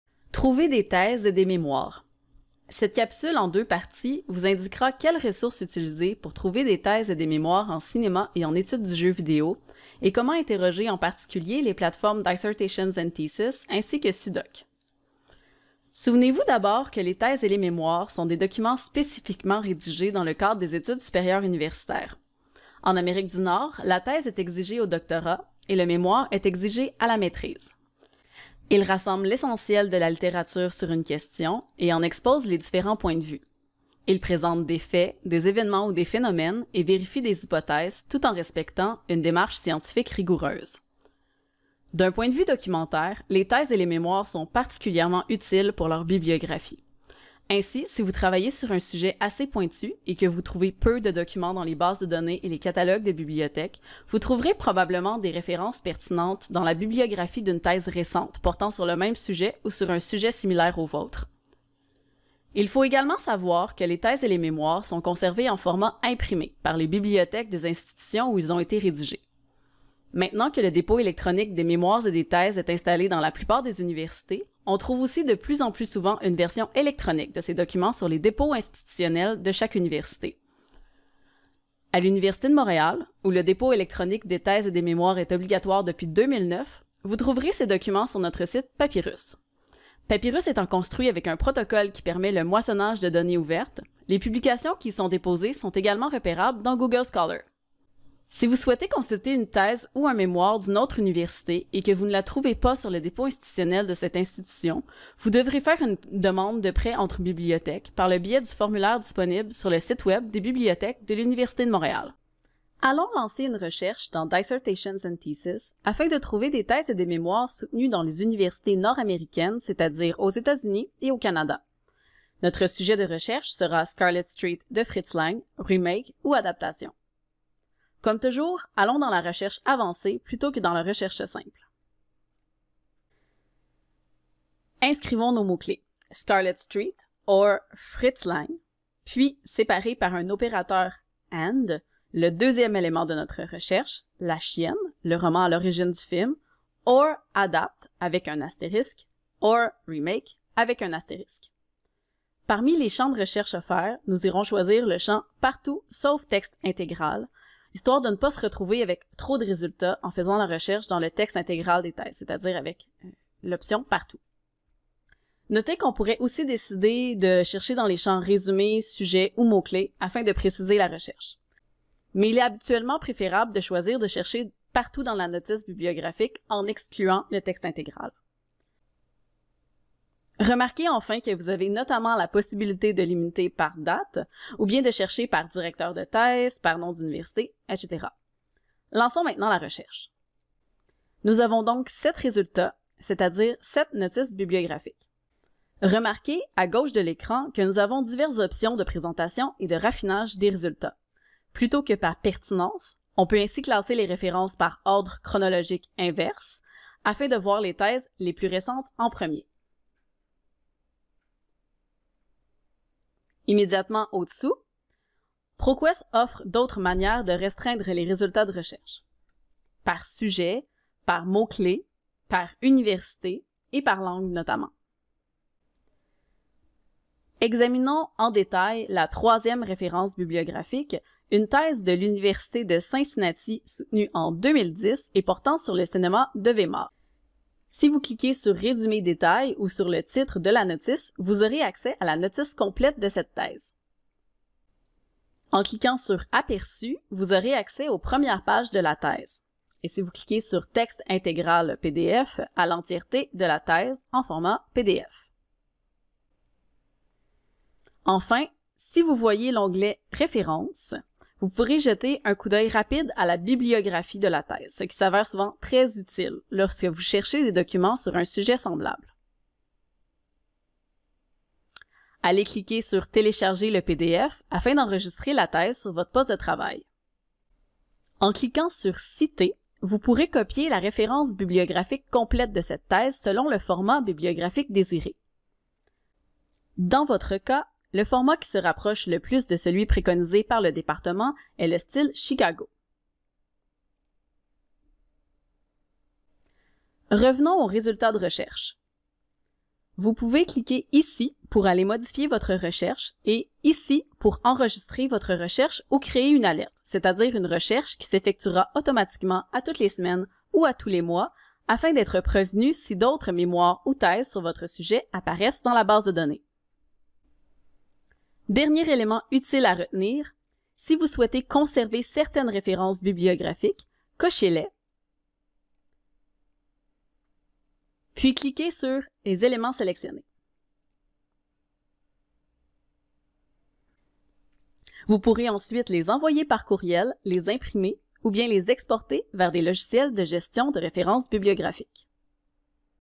CIN1104 - Trouver des thèses et des mémoires, partie 1
Cette capsule en deux parties s'adresse aux étudiants du cours CIN1104 Culture et documentation du cinéma. Vous y apprendrez quelles ressources utiliser pour trouver des thèses et des mémoires et comment interroger en particulier la base de données Dissertations and Theses de ProQuest ainsi que le catalogue français SUDOC.